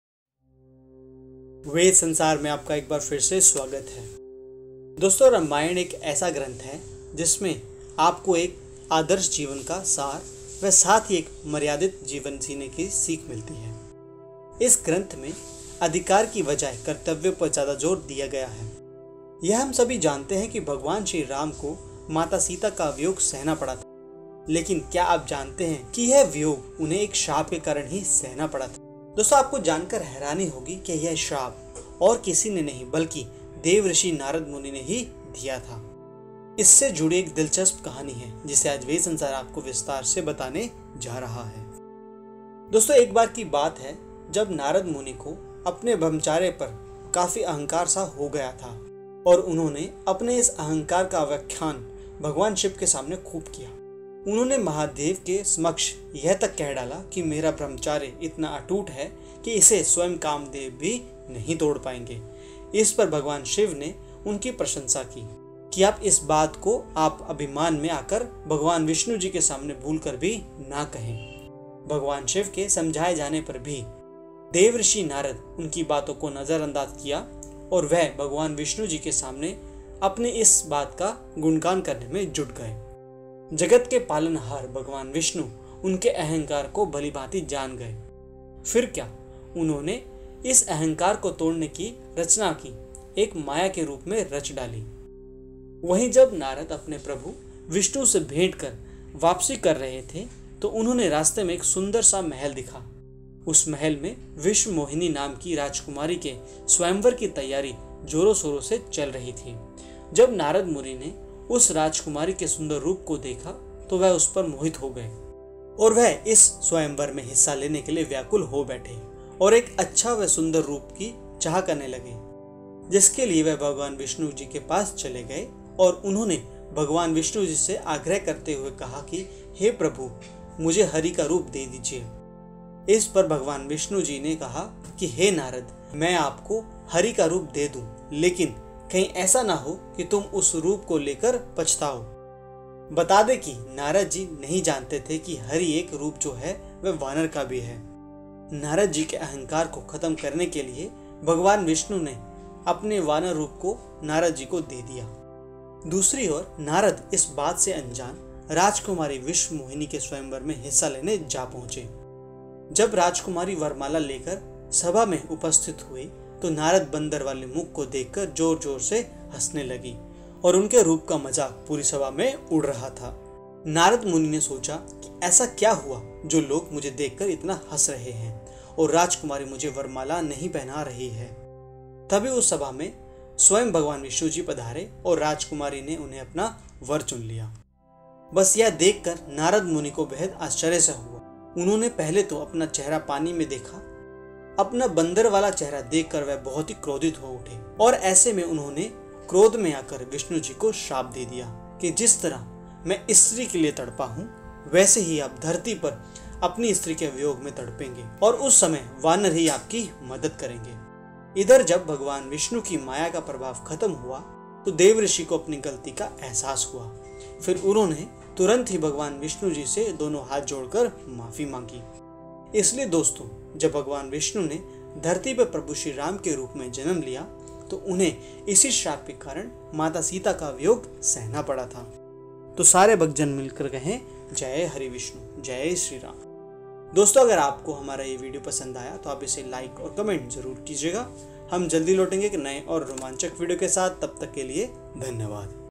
जब नारद मुनि ने भगवान विष्णु को दिया ये श्राप
रामायण एक ऐसा ग्रंथ है जिसमें आपको एक आदर्श जीवन का सार व साथ ही एक मर्यादित जीवन जीने की सीख मिलेगी। इस ग्रंथ में अधिकार की बजाय कर्तव्यों पर ज्यादा जोर दिया गया है। यह हम सभी जानते हैं कि भगवान श्रीराम को माता सीता का वियोग सहना पड़ा था… लेकिन क्या आप जानते हैं कि यह वियोग उन्हें एक श्राप के कारण ही सहना पड़ा था।

आपको जानकर हैरानी होगी कि यह श्राप और किसी ने नहीं बल्कि देव ऋषि नारद मुनि ने ही दिया था। इससे जुड़ी एक दिलचस्प कहानी है… जिसे आज वेद संसार आपको विस्तार से बताने जा रहा है –

एकबार की बात है जब नारद मुनि को अपने ब्रह्मचर्य पर काफी अहंकार सा हो गया था और उन्होंने अपने इस अहंकार का बयान भगवान शिव के सामने खूब किया। उन्होंने महादेव के समक्ष यह कह डाला कि – “मेरा ब्रह्मचर्य इतना अटूट है कि इसे स्वयं कामदेव भी नहीं तोड़ पाएंगे…” इस पर भगवान शिव ने उनकी प्रशंसा की, परंतु उन्हें इस बात को लेकर भी चेताया कि इस बात को आप अभिमान में आकर भगवान विष्णु जी के सामने भूल से भी ना कहें।

इधर जब भगवान विष्णु की माया का प्रभाव खत्म हो गया तो देवर्षि को अपनी गलती का अहसास हुआ… फिर उन्होंने तुरंत ही भगवान विष्णु जी से दोनों हाथ जोड़कर क्षमा मांगी।

इसलिए दोस्तों, जब भगवान विष्णु ने धरती पर प्रभु श्रीराम के रूप में जन्म लिया तो उन्हें इसी श्राप के कारण माता का वियोग सहना पड़ा था।